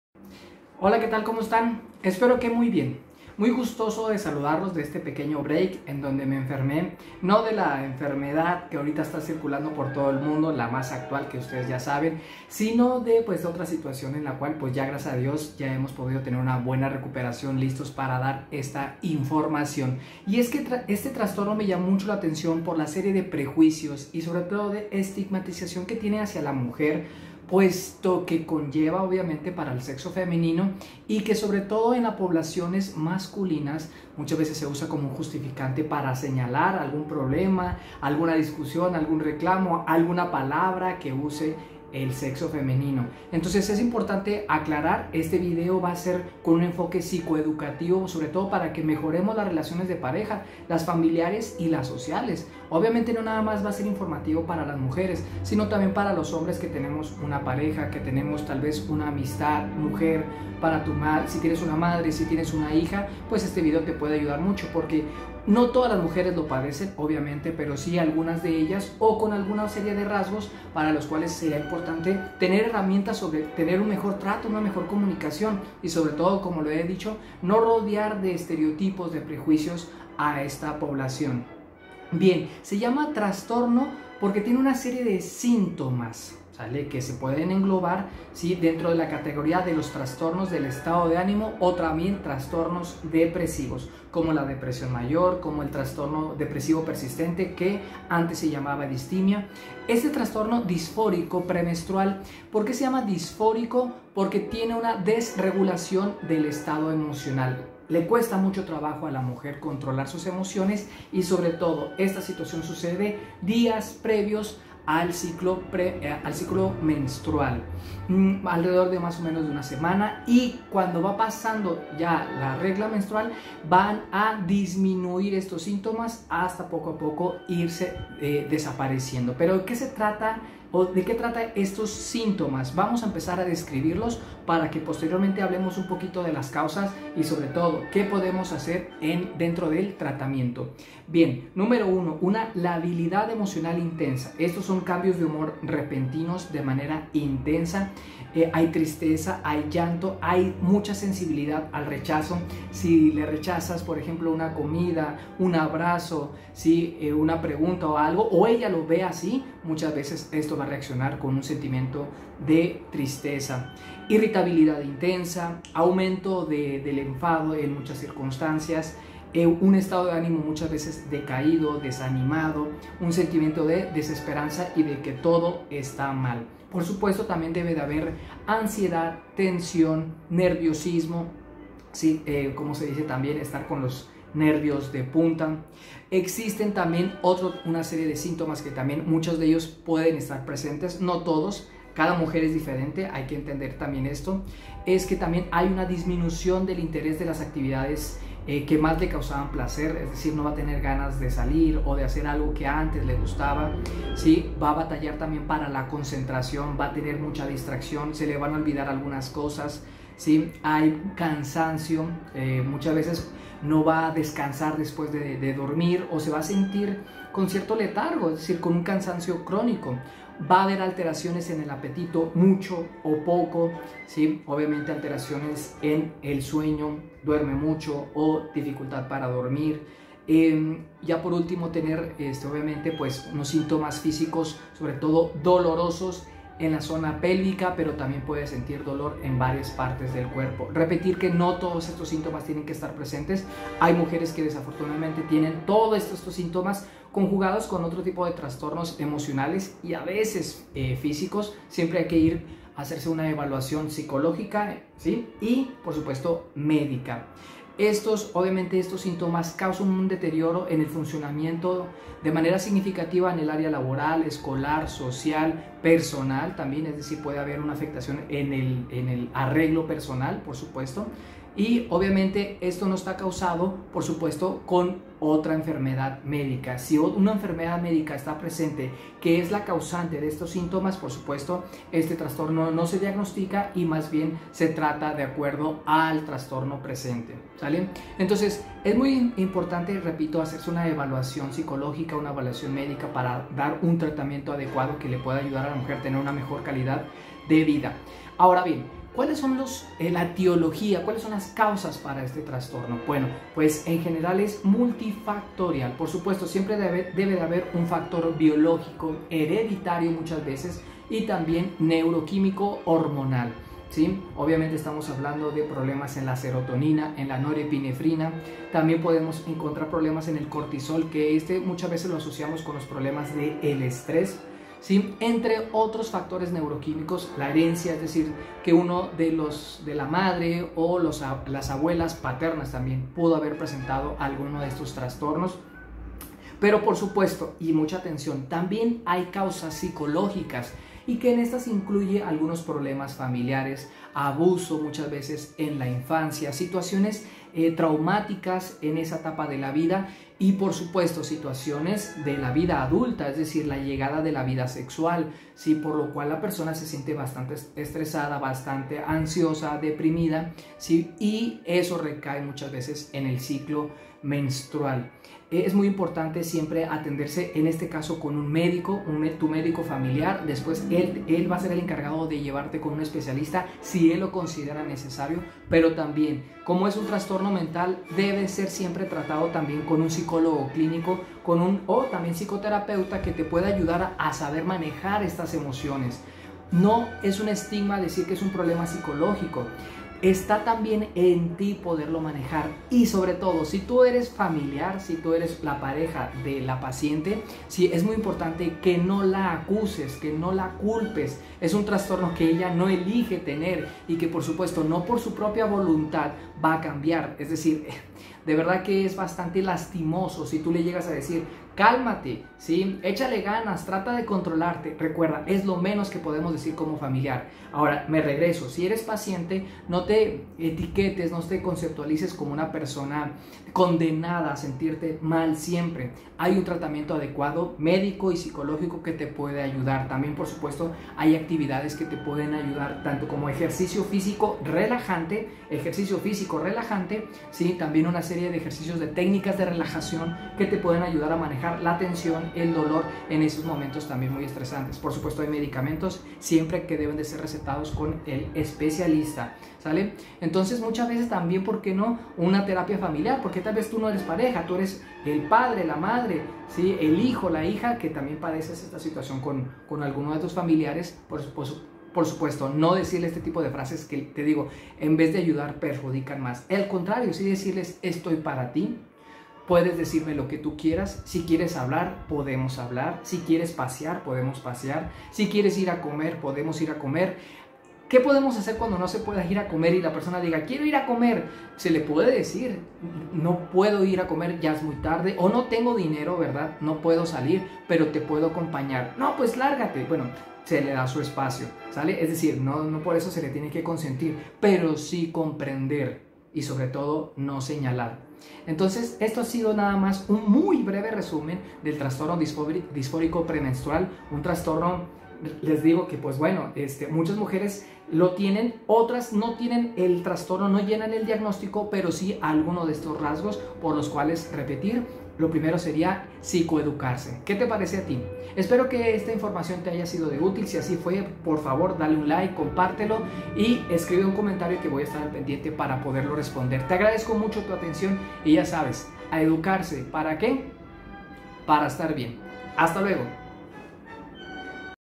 El Trastorno Disfórico Premenstrual "¿Estas en tus días? ¿Qué tienes?... nada"
Forma parte de los Trastornos Depresivos, le sucede a algunas mujeres teniendo varios factores hormonales, neuroquímicos y psicosociales, existe un tratamiento que puede ayudar a mejorar la calidad de vida entre ellos el tratamiento medicamentoso, la psicoterapia y la orientación a la familia de la paciente, como la evitación de comentarios prejuiciosos. Información tomada del DSM- V. Gracias por tu atención y SUSCRIPCIÓN!